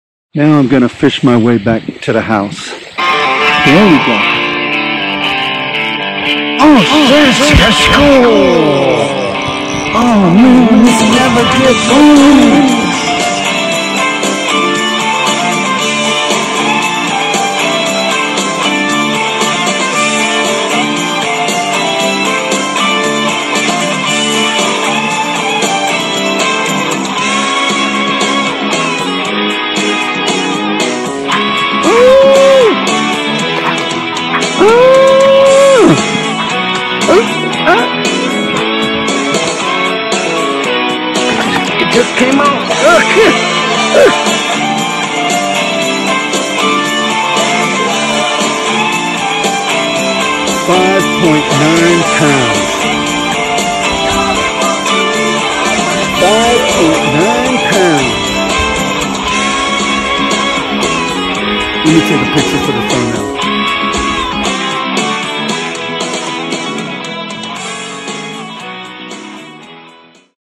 Personal Best Short Film!!
This is a short version of the longer film that centers around my personal best Bass catch up to this point. of course we all hope to best our personal best! So I'll endeavor to do the same! We hope you enjoy these videos as we have committed to donating all profits to causes once the channel is monetized. so with that, thank you for subscribing and assisting others!!